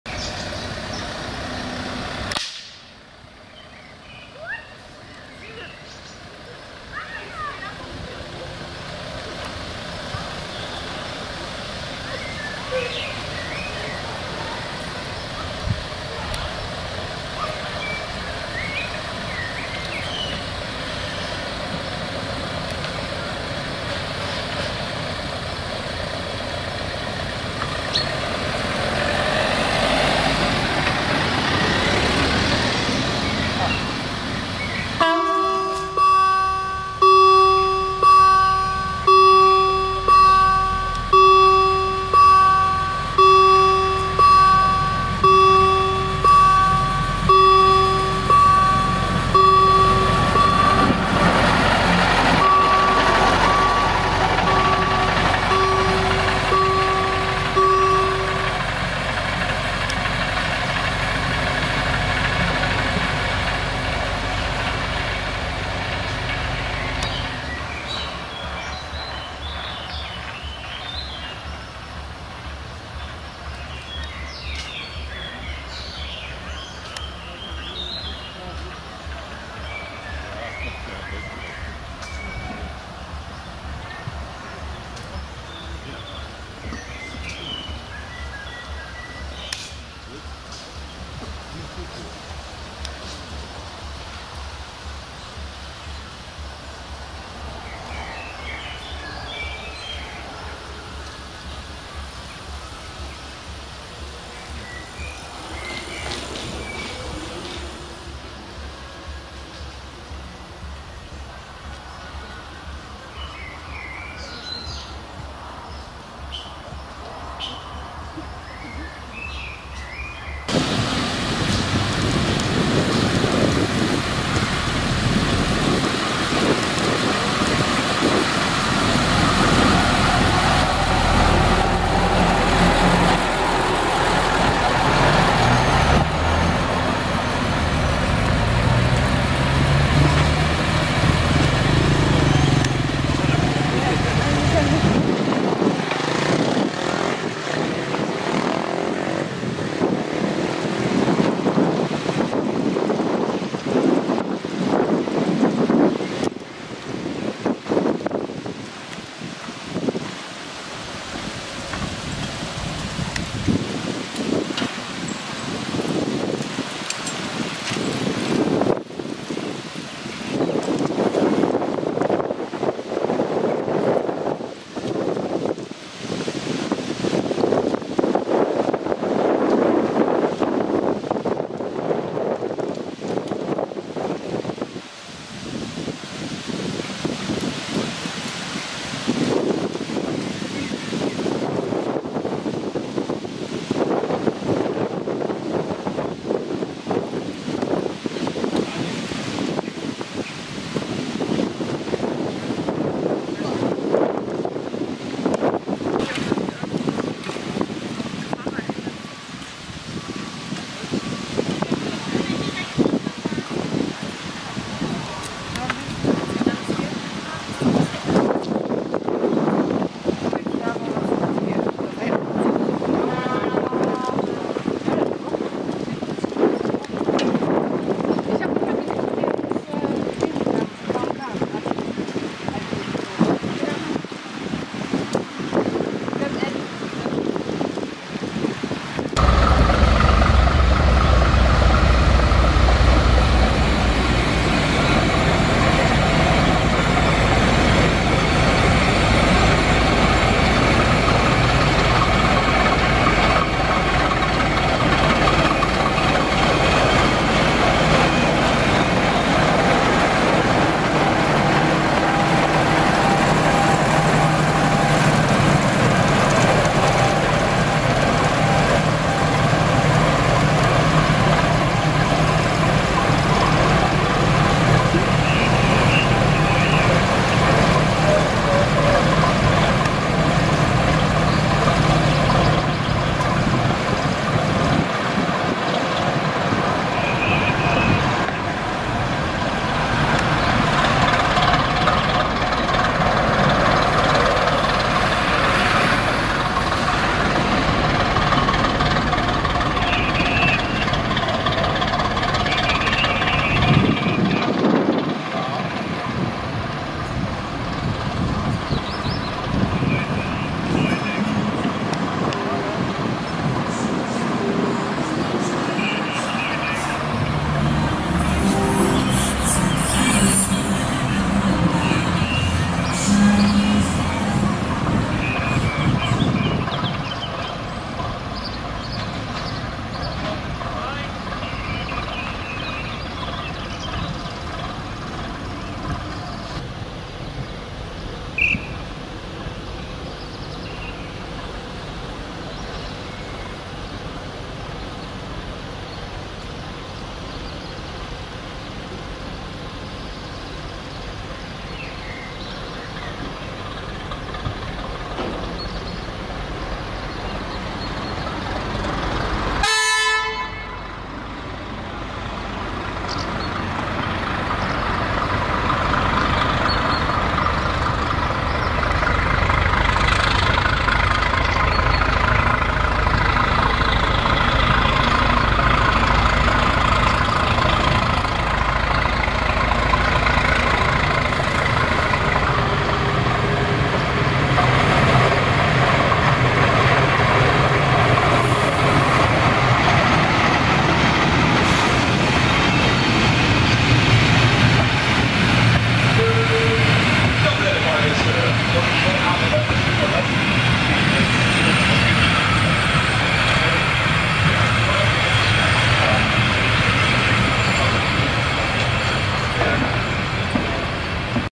Spoorweg bedrijf in Bad Essen aan Wittlager Buurtspoor bij Osnabruck
Er is een sterke werking in Bad Essen op deze zondag, de 5.Juni.2016. Zowel de stoom enthousiastelingen Osnabrück met zijn V65 als het museum spoor Minden met hun vintage treinstellen bezetten de weinige spooren.